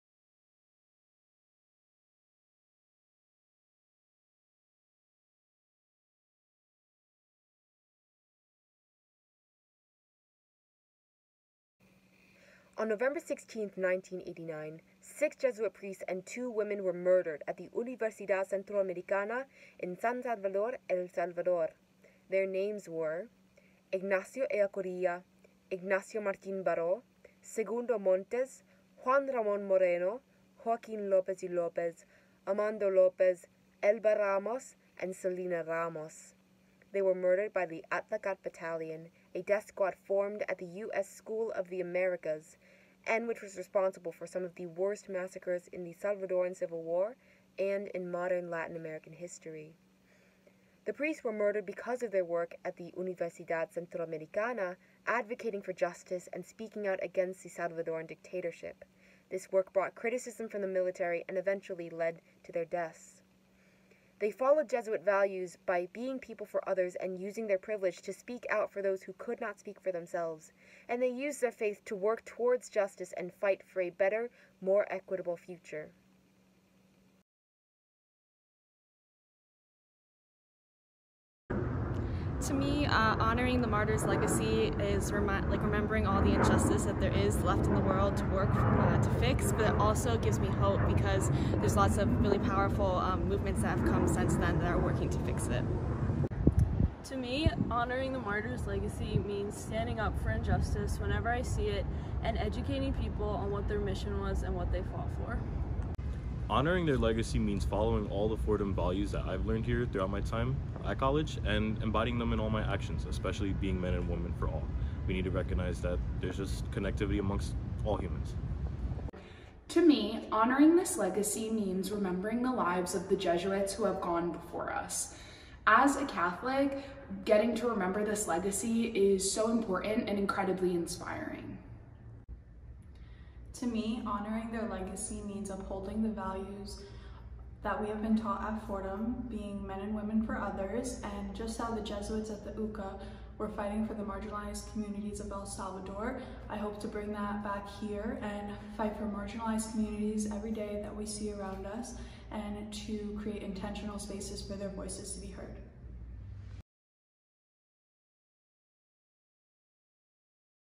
The Jesuit Martyrs of El Salvador
Members of the Fordham community commemorate the legacy of six Jesuit priests and two women killed on November 16, 1989 at the University of Central America in El Salvador.